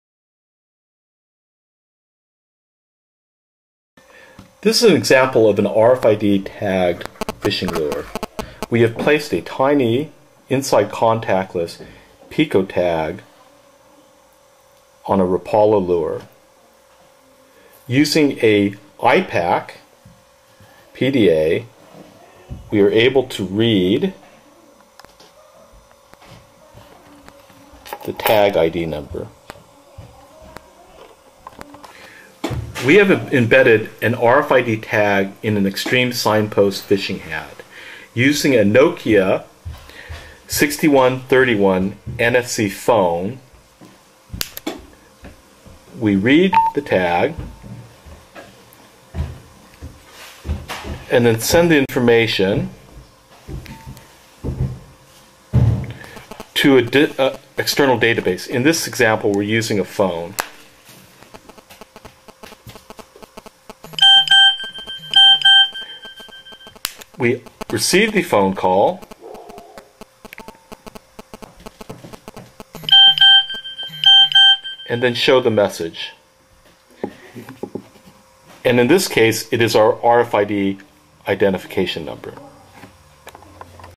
Xtreme Tagging Applications for fishing (E0070000021BF365)
We have the ability to leave electronic messages on fishing tackle and hats using modified cell phones and RFID tags.  In this example we have attached a small Inside Contactless RFID tag to the lip of a Rapala lure. We wrote information on the tag using an IPAQ PDA and a Tradewinds SDIO reader.